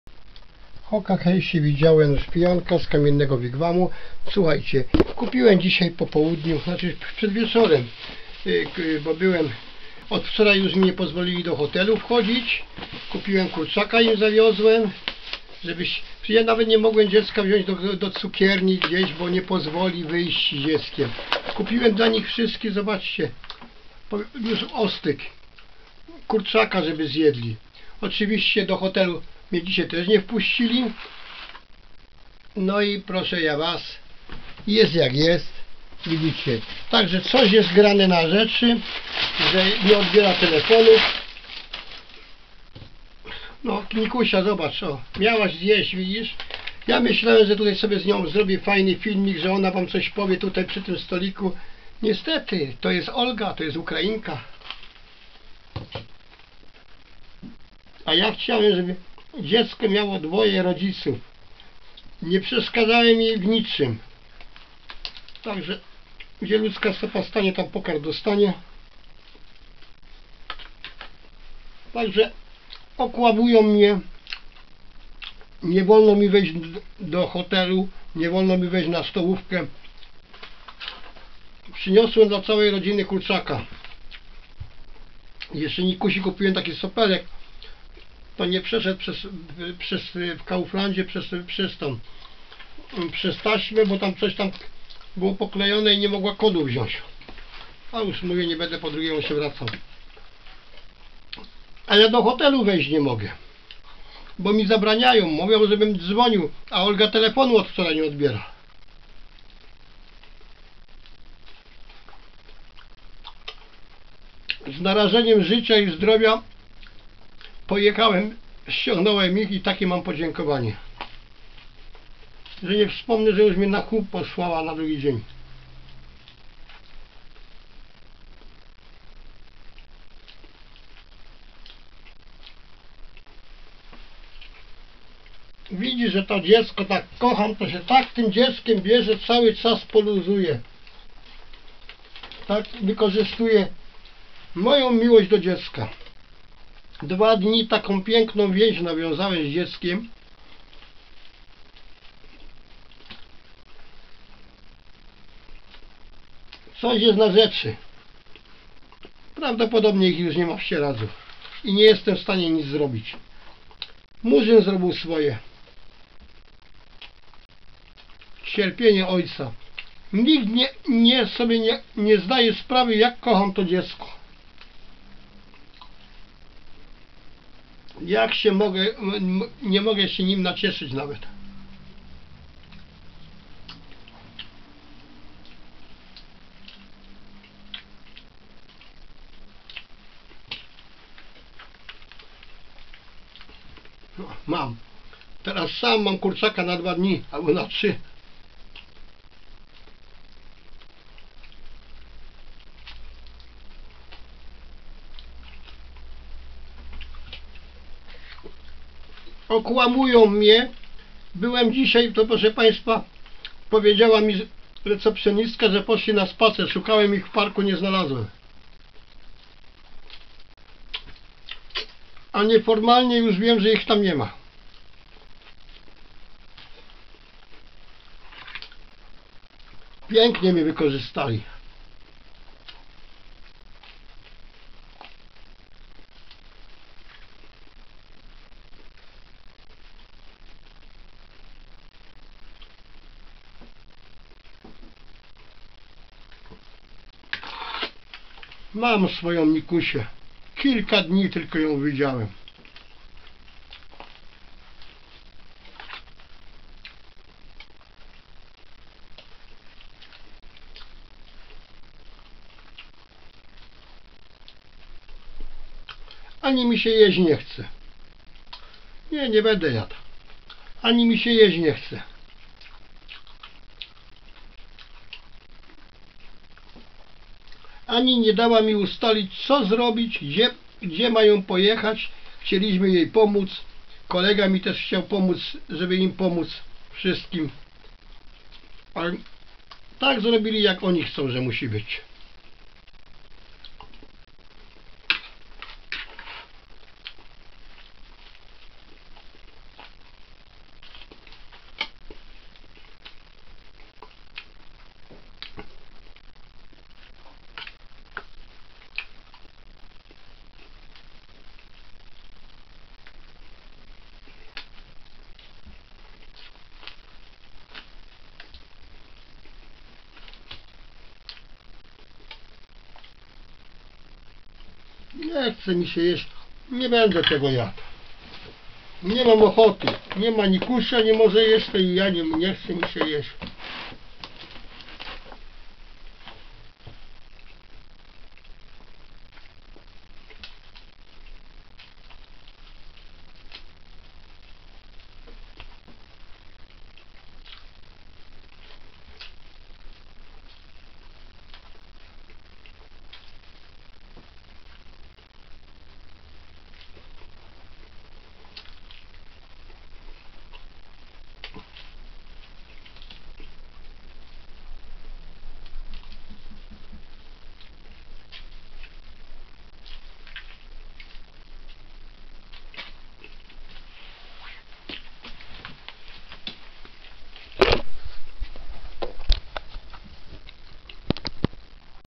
DSCF0394 SIERADZ Nic juz mnie nie cieszy Nawet Corce kurczaka nie moglem dac ani z nia zjesc w barze
Juz...czuje ze stalo sie!Ich juz w Sieradzu nie ma!W Hotelu od wczoraj oklamuja mnie dzis recepcjonistka oklamala mnie ze wyszli na spacer a ich juz tam nie ma!Misja zakonczona...porazka!Nie bede juz chyba nic krecil popadam przez to w depresje...;Murzyn zrobil swoje...i juz jest nie potrzebny!Dlaczego Zycie takie jest?!